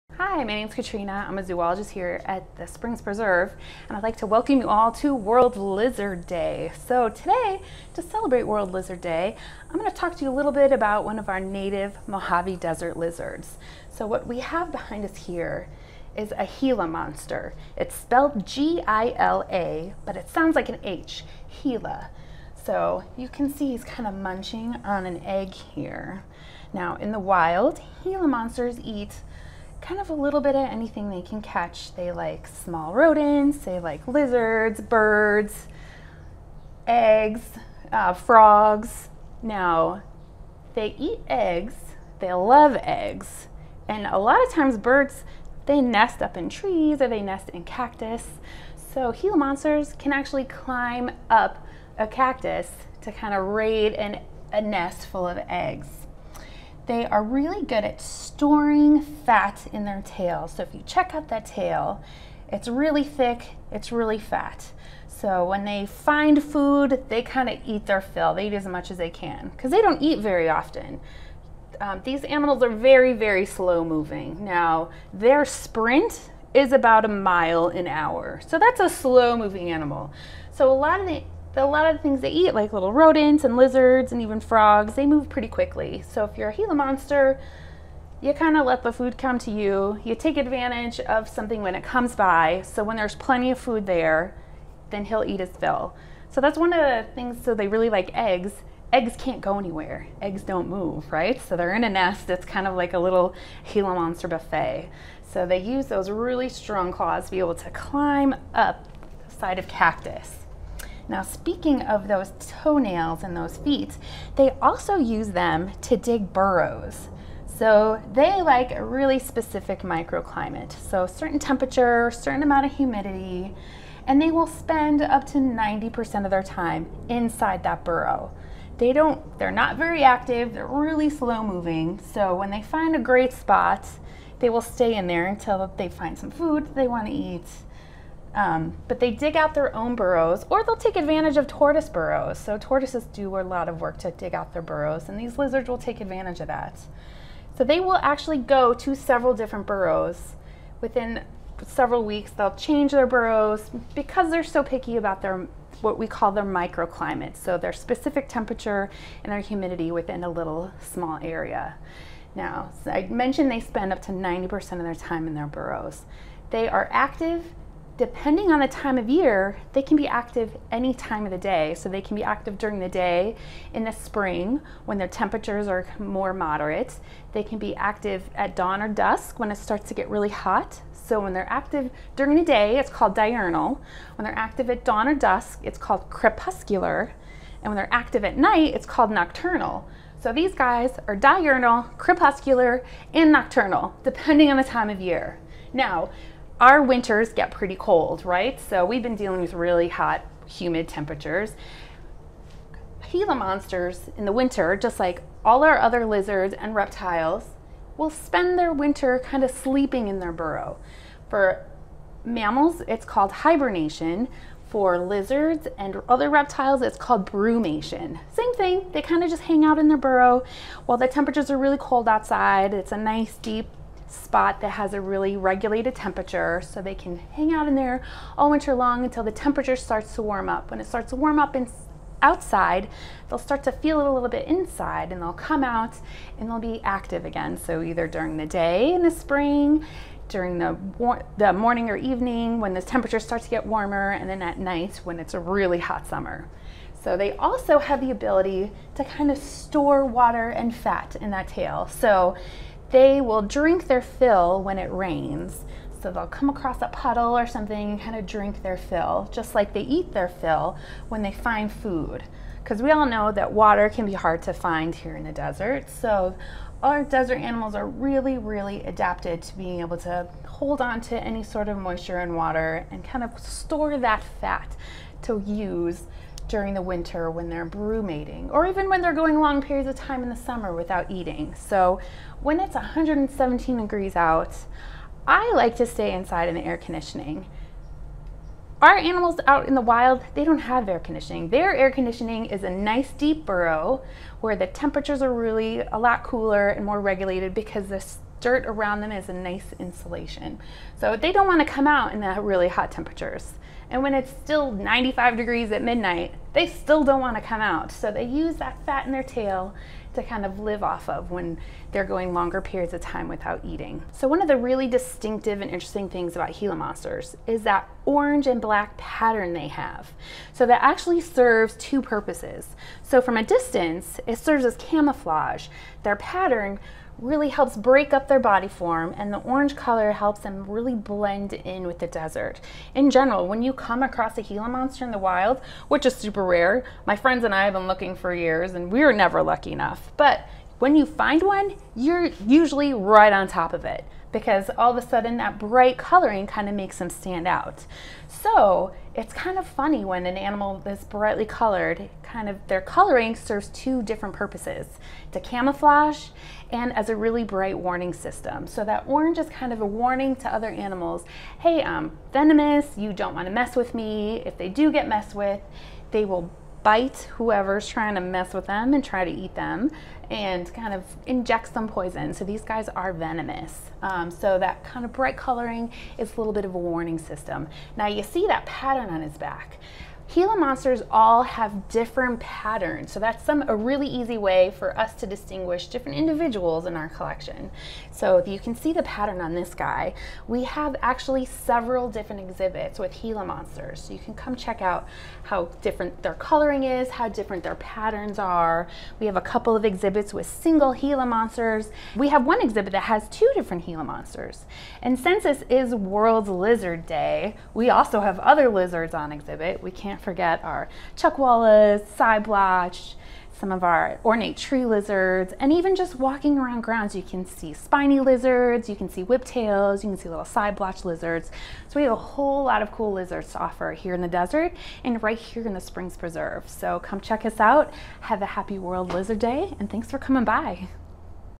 Learning About Gila Monsters | World Lizard Day
Hey, lizard lovers! 🦎 Tune in with our zoologist for an up-close look at our resident venomous lizard—the Gila monster!